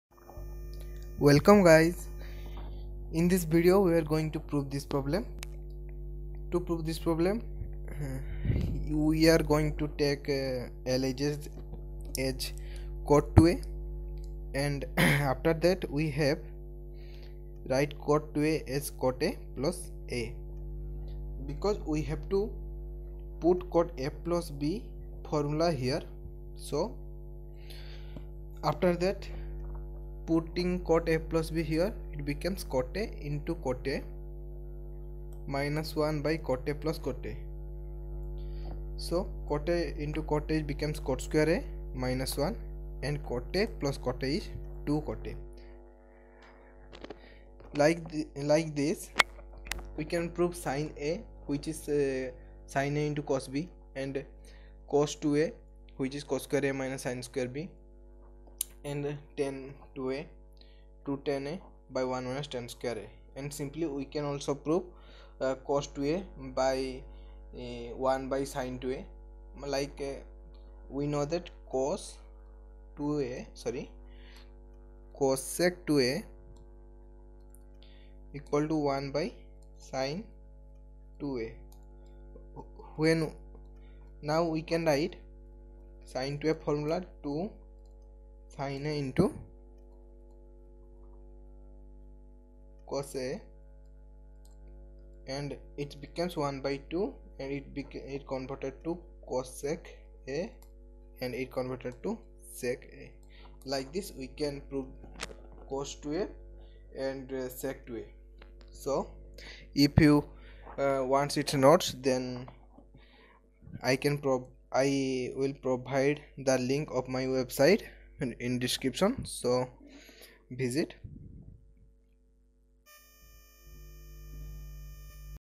Prove that cot2A=cot^2A-1/2cotA
To prove that cot2A=cot^2A-1/2cotA

1.we have to take L.H.S as cot2A
2. After that write cot2A inform of cot(A+A)
3.Put the formula of cot(A+B) formula in cot(A+A)
4.on solving this equation we get our required R.H.S
Hence we proved cot2A=cot^2A-1/2cotA